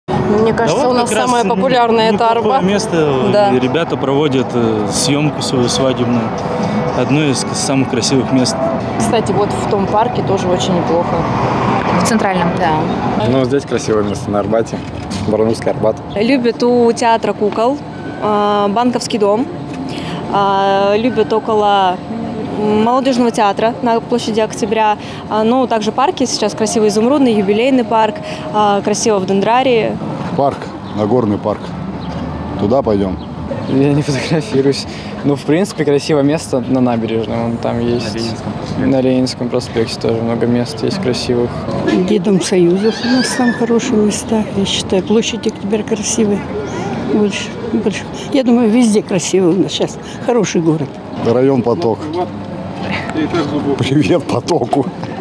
Опрос: барнаульцы рассказали, где городе можно сделать красивые кадры
Накануне главного городского праздника редакция телеканала "Толк" продолжает опрашивать жителей столицы Алтайского края .
До конца жаркого сезона остается меньше месяца. Если вы хотите запечатлеть себя и своих близких в красивых локациях города – вот вам рекомендации о барнаульцев, где это лучше сделать.